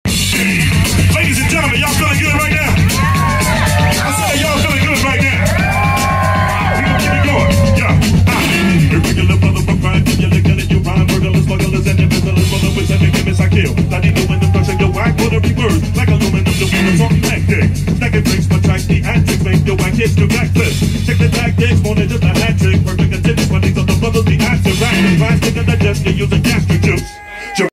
Chali2na and Cut Chemist at Raindance Campout 2022
Chali2na and Cut Chemist killing it. jurassic5 raindancecampout oldschoolhiphop pixel6 @J5Official